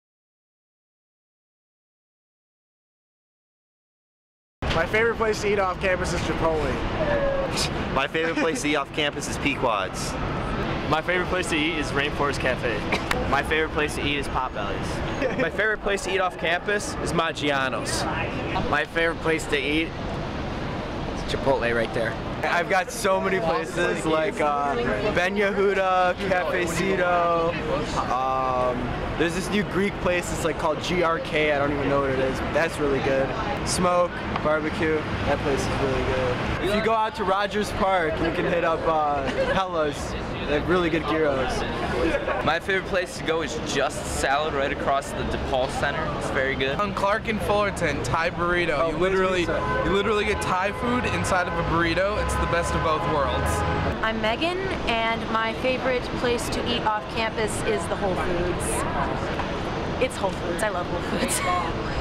POLL: What is your favorite off-campus restaurant?
DePaul students tell the DePaulia where they like to eat off campus.